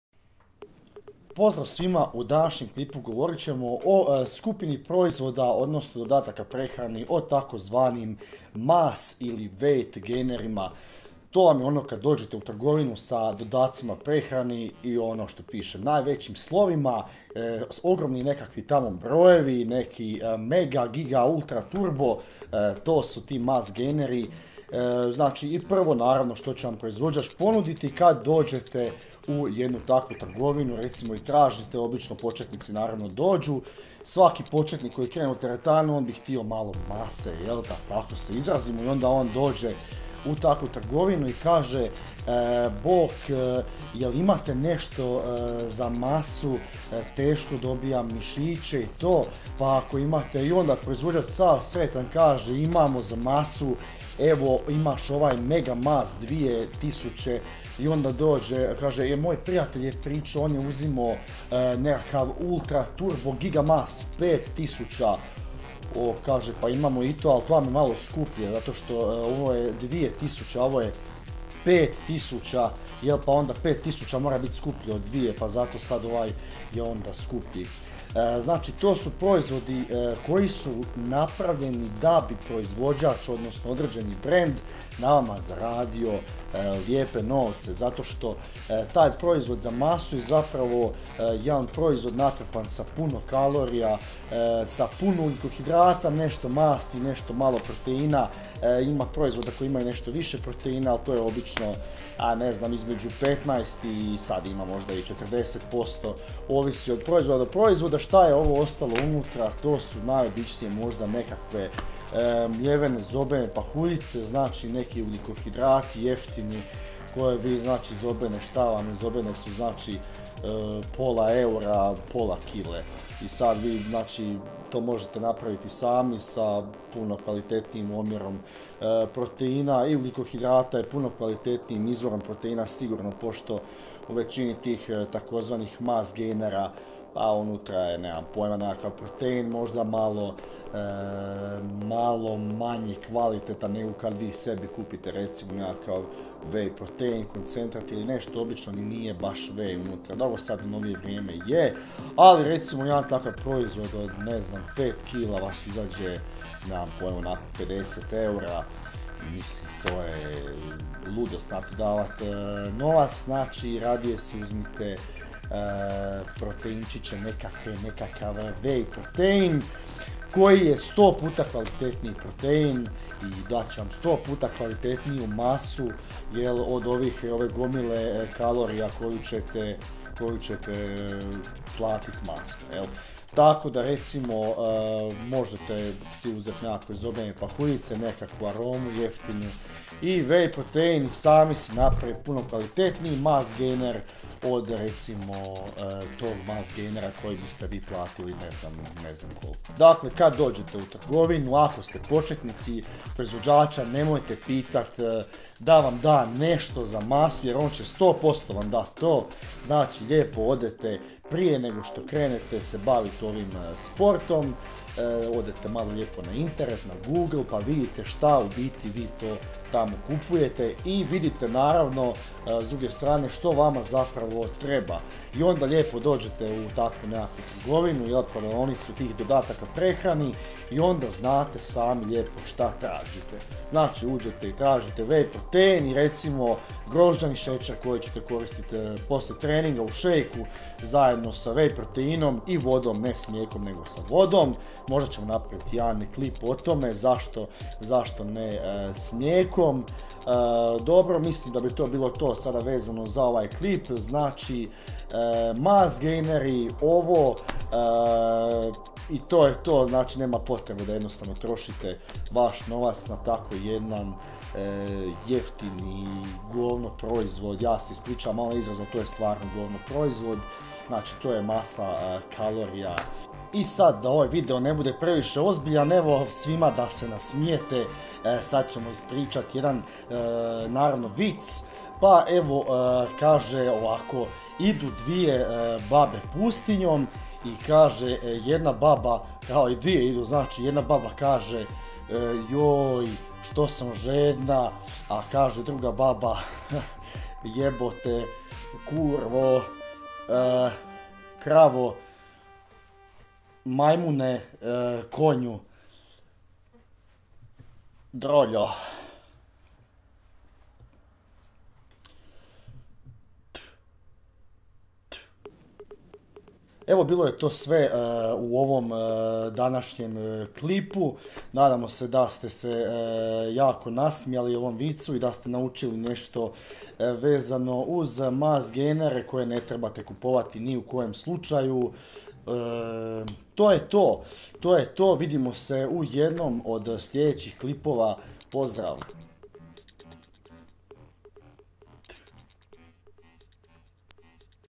MASS GAINERI
U ovom klipu odgovaramo na pitanje što su to mass gaineri i jesu li neophodni.